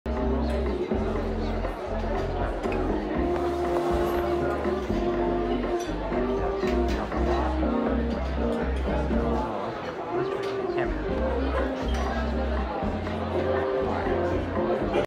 Si estás dispuesto a esperar, tiene que ser HEINZ.
⏳ Ya viene...A veces tarda, pero llega. Aunque mires el celular inquieto o trates de hacer contacto visual con el mesero, crecen las ganas de disfrutar del sabor de Heinz 🤤. Porque cuando el 84% confirma haber esperado por las salsas que le dan un sabor único a la comida